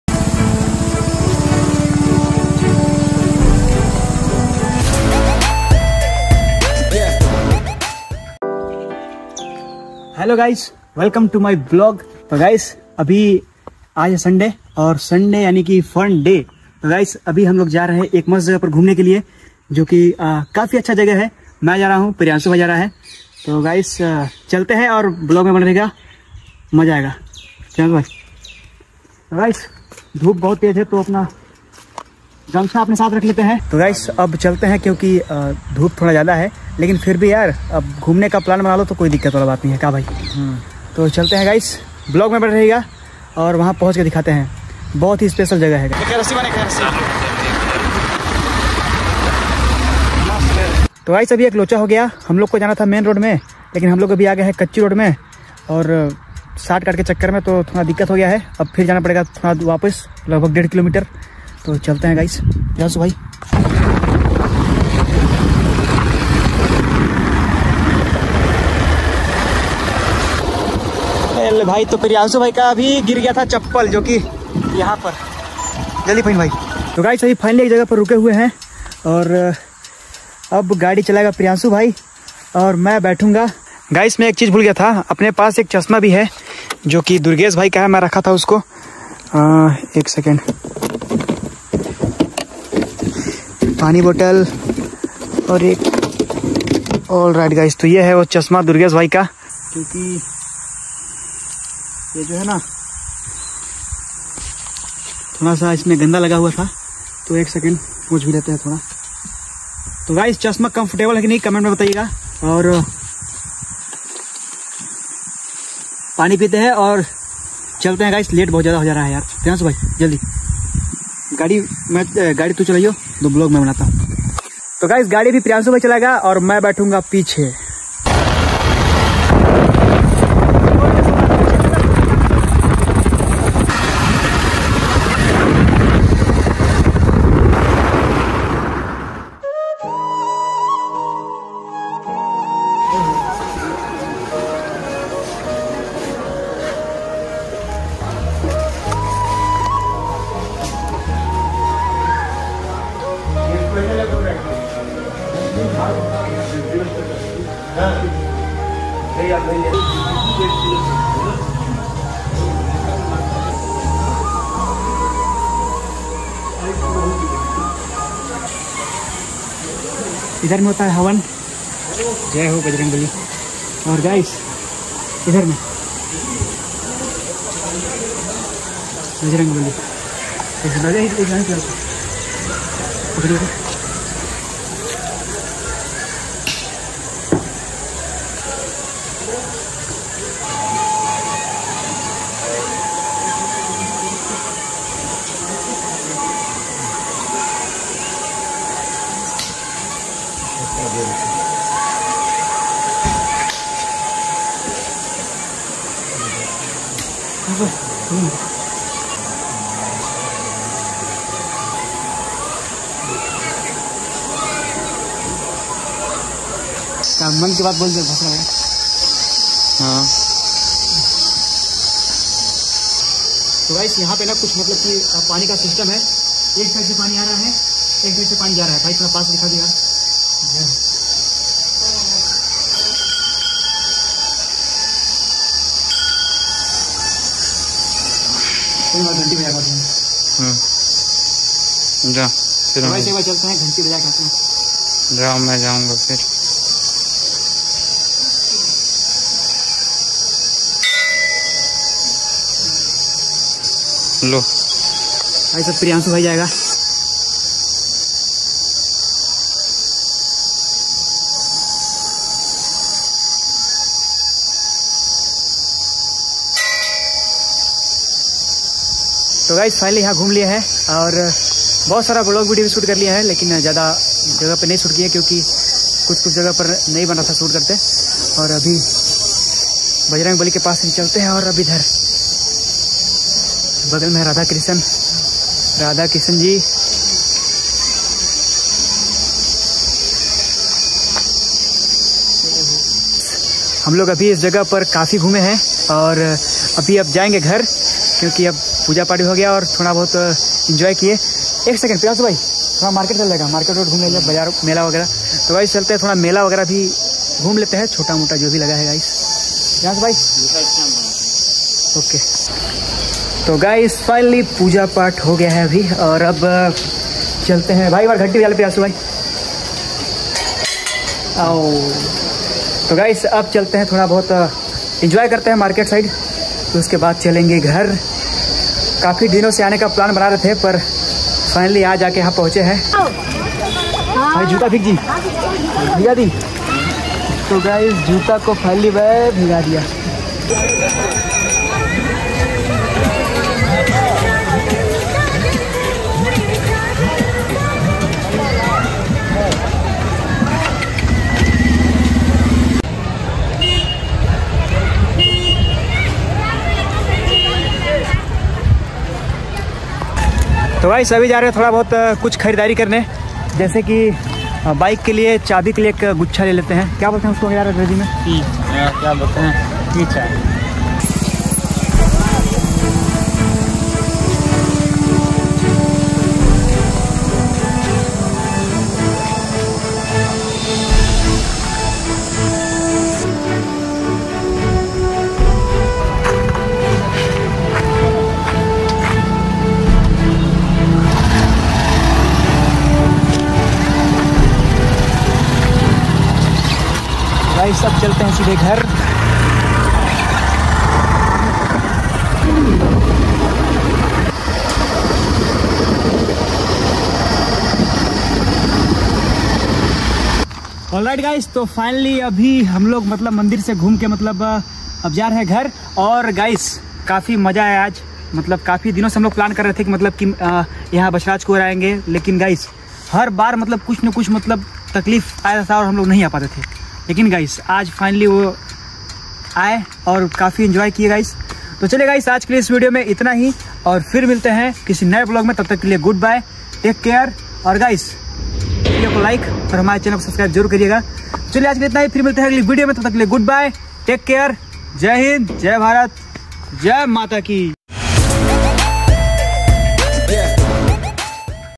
बाबा बछराज कुंवर VLOG ( Balrampur )
यह वीडियो बाबा बछराज कुंवर धाम का है जो कि हमारे ग्राम पंचायत से लगभग 50 किलोमीटर दूर है

छत्तीसगढ़ राज्य के बलरामपुर जिले में स्थित बाबा बछराज कुंवर धाम जो कि अपने आप में मानत है। यहां पर हर दिन भक्तों का भीड़ काफी ज्यादा रहता है।

मैं और मेरा दोस्त प्रियांशु भाई आज हम दोनों गए थे बाबा बछराज कुंवर धाम जो की काफी अच्छा लगा, आज का दिन हमारा बहुत अच्छा रहा

चलिए दोस्तों आज की इस वीडियो में इतना ही अगर आप हमारे चैनल पर न्यू है तो हमारे चैनल को सब्सक्राइब करें वीडियो को लाइक और अपने दोस्तों को शेयर भी जरूर करें।